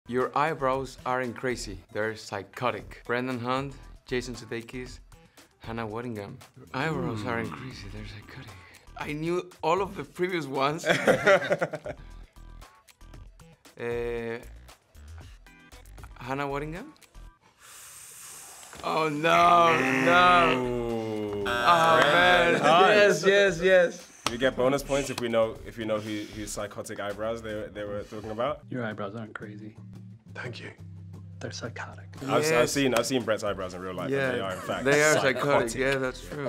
Which 'Ted Lasso' Cast Member Remembers The Most Lines From Season 1 & 2? | Who Said That? | ELLE
'Ted Lasso' is a TV favorite, and there are too many iconic lines to name, making this episode of 'Who Said That' extra tricky.

Watch Season 3 of 'Ted Lasso' on Apple TV+.